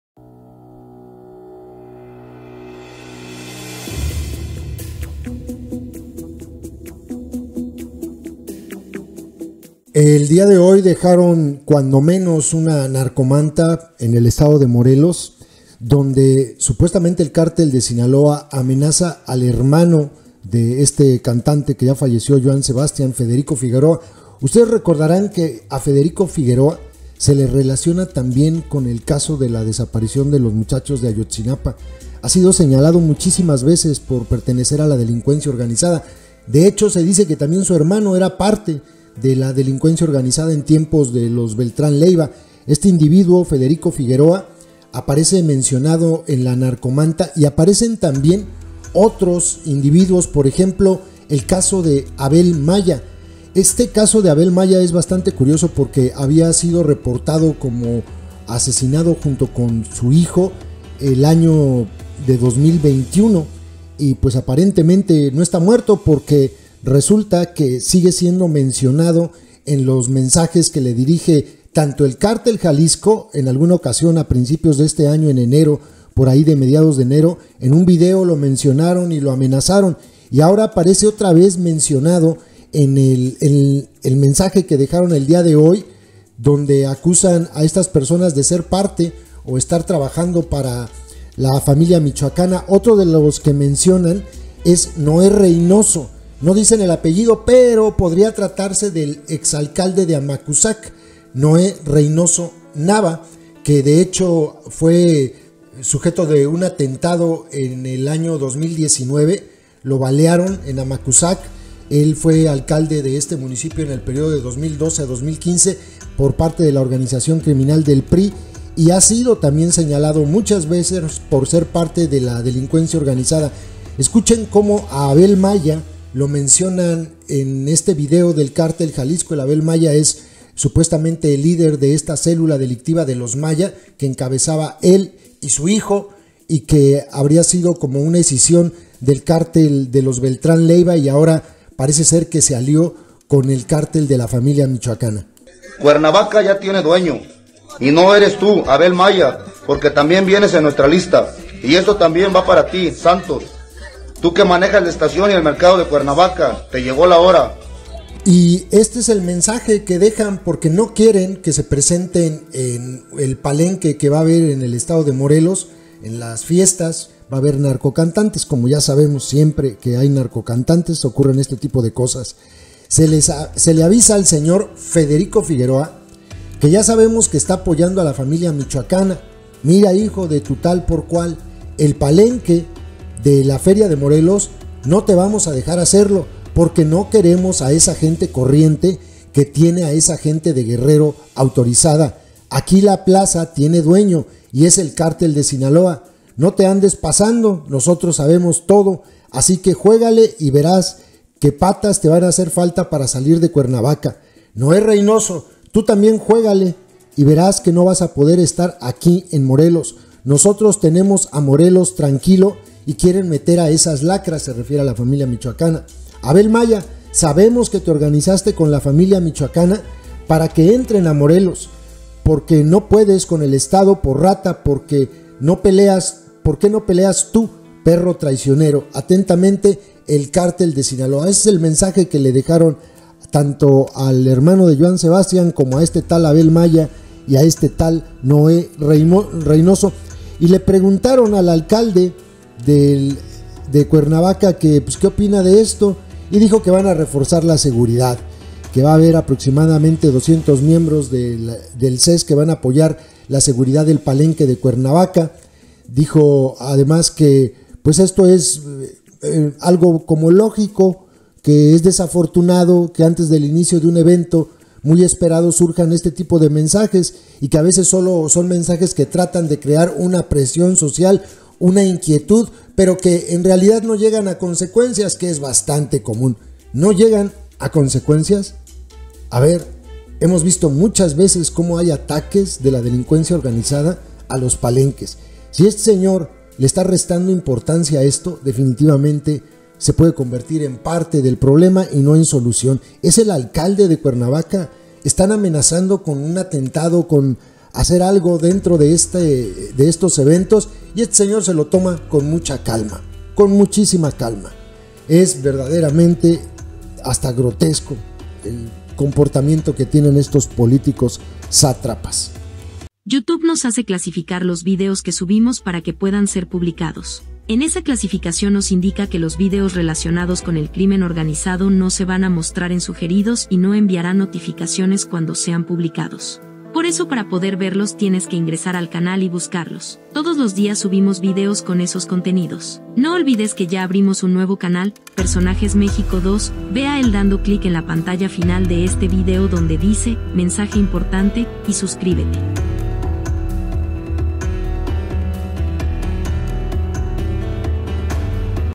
Cártel de Sinaloa advierte a Federico Figueroa que no permitirán hacer el Palenque de Cuernavaca
En Cuernavaca, el día de hoy el Cártel de Sinaloa dejó un mensaje en el que advierte a Federico Figueroa, hermano del fallecido Joan Sebastian, así como a Abel Maya y a Noé Reynoso, que no permitirán que se lleve a cabo el  palenque de la ciudad. Señalan que los tres están aliados a la Familia Michoacana y que esa es la razón por la que no permitirán que ocurra el evento.